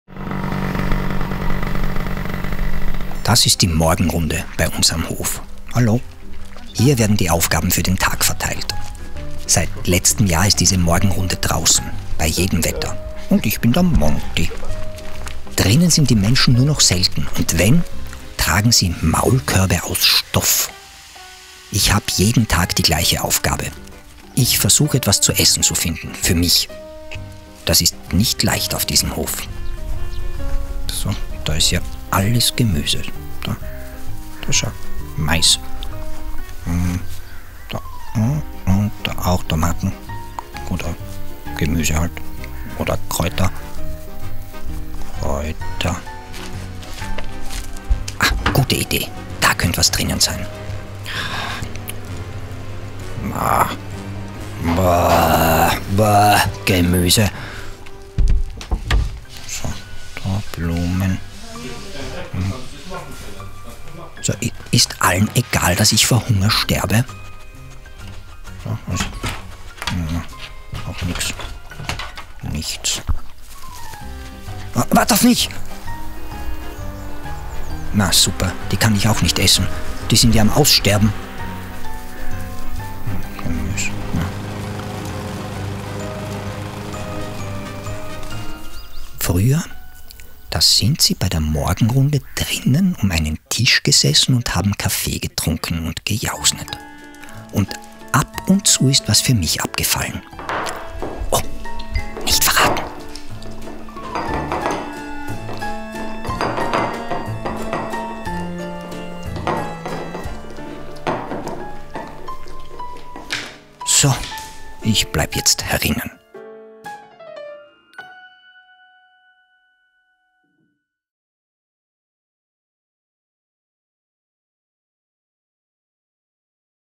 Wie Jugend am Werk Steiermark die Corona-Pandemie meistert. (Folge 1 von 7)
Als Unternehmen der Sozialwirtschaft trägt Jugend am Werk Steiermark gesellschaftliche Verantwortung und ist ein verlässlicher Partner. Auch oder besonders in Zeiten der Quarantänen, Kurven, Herden, Tests, Masken, Verordnungen, Risikogruppen, Impfungen, Schutzmaßnahmen, Lockdowns, Video-Meetings, Desinfektionsmittel, Inzidenzen, Vulnerabilitäten, Zutrittsbeschränkungen …

Wie wir die Herausforderungen meistern zeigen nun sieben Videos*.

Best Practice: Ökologische Landwirtschaft Attendorf
Unsere Ökologische Landwirtschaft Attendorf macht aufgrund von Corona die Morgenbesprechung immer draußen, bei Schnee, Regen, Wind, Sonne etc. werden jeden Morgen die Pläne für den Tag besprochen.

*Vielen Dank an das Team vom Theater im Bahnhof für die Idee und Ausführung.